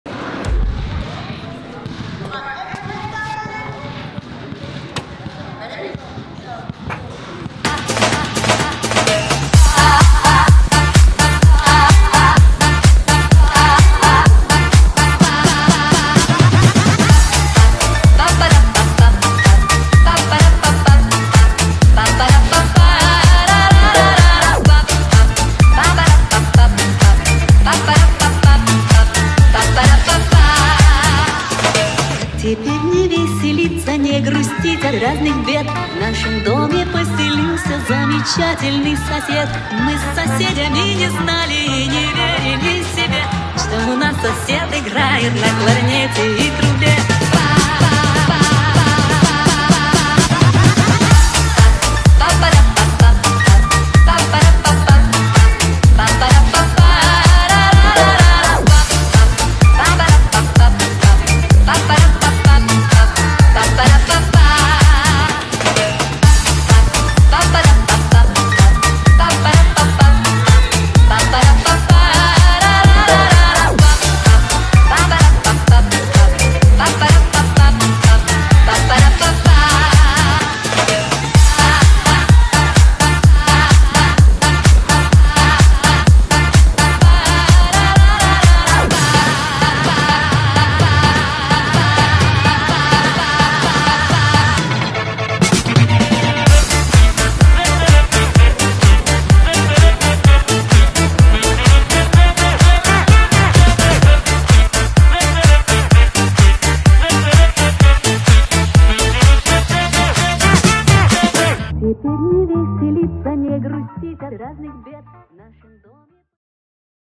Celebrating International Women's Day with a special gymnastics showcase!
Participants from our Community Engagement gymnastics session, have been working on a routine to celebrate International Women's Day.